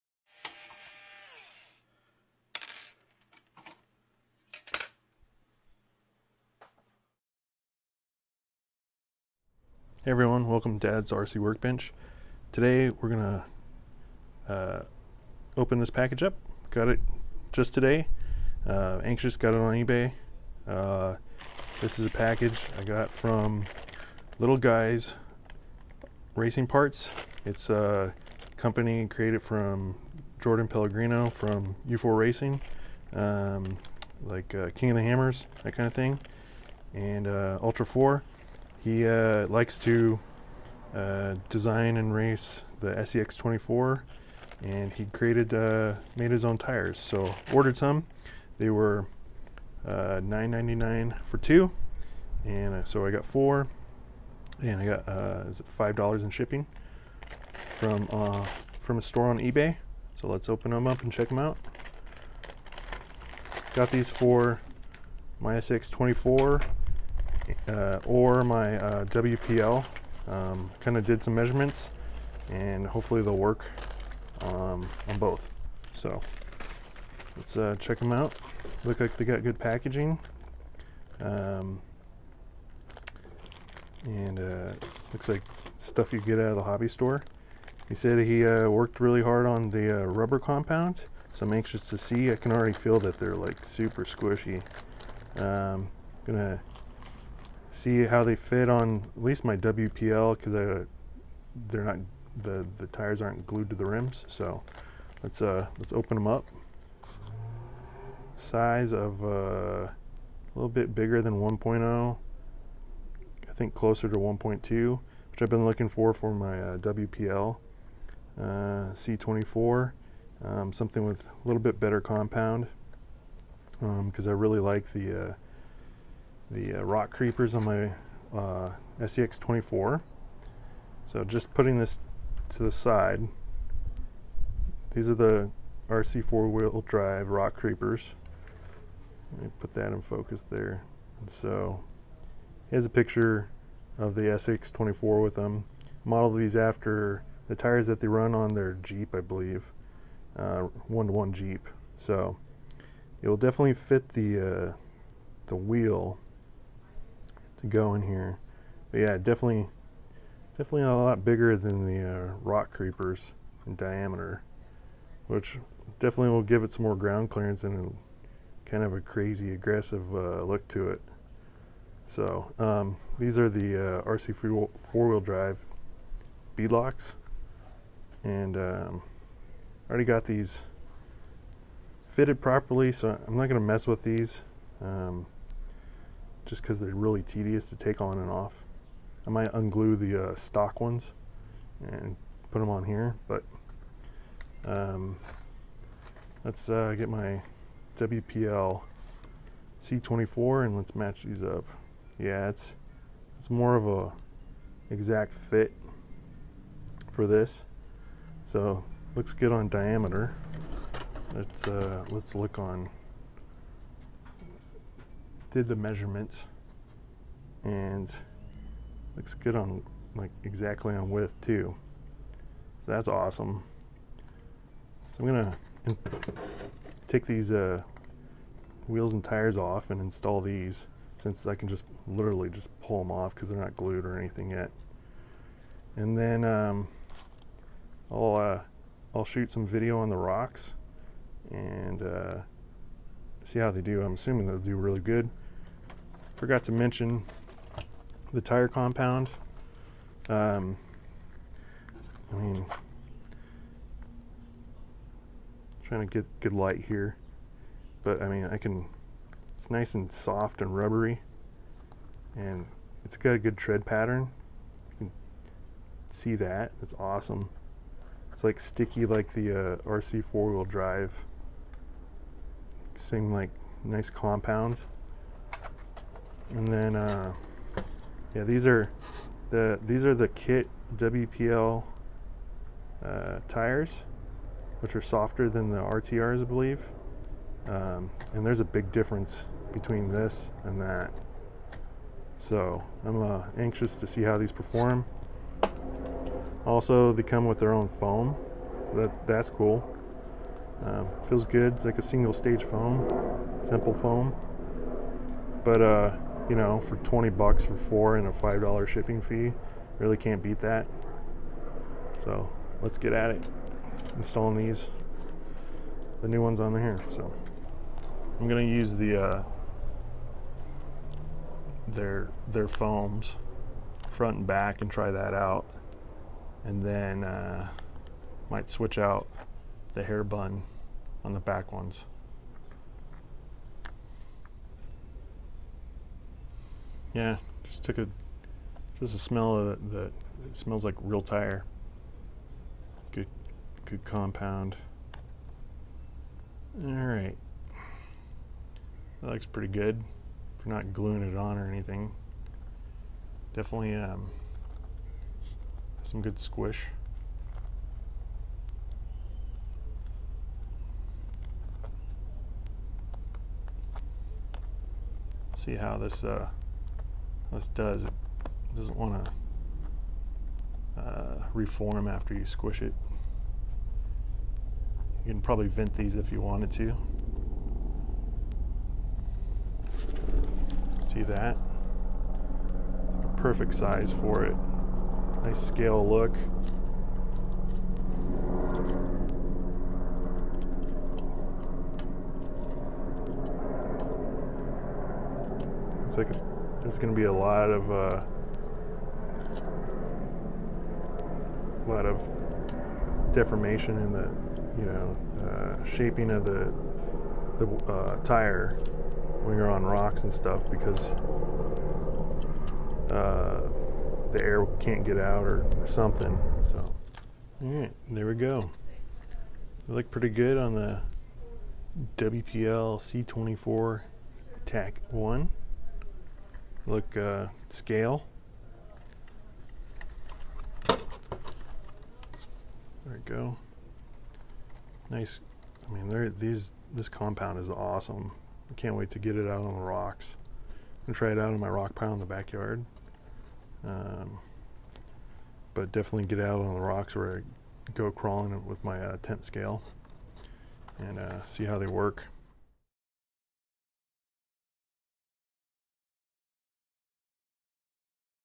Little Guy Racing Parts 1.2 Tires Review for WPL C24-1 or Axial SCX24
I received Little Guy Racing Parts 1.2 tires in the mail and installed them on my WPL C24-1 instead of my Axial SCX24 due to size and they were easier to install on my C24.  The price, rubber compound, and size were all great features of these tires.

Intro audio from alienistcog on Freesound